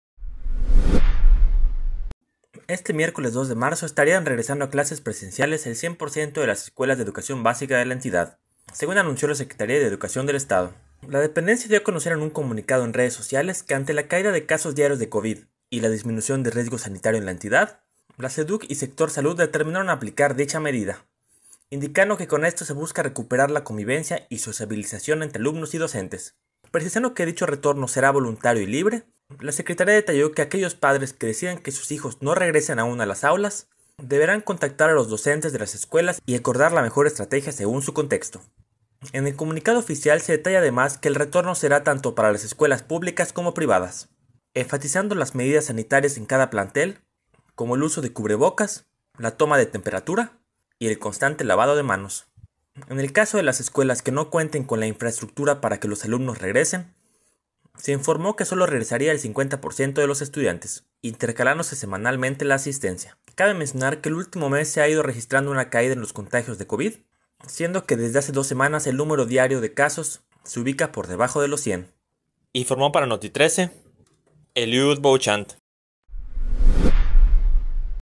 VideoN🎥|Este miércoles 2 de marzo estarían regresando a clases presenciales el 100% de las escuelas
VideoNota 🎥 | Este miércoles 2 de marzo estarían regresando a clases presenciales el 100% de las escuelas de Educación Básica de la entidad, anunció la Secretaría de Educación del Estado (Seduc).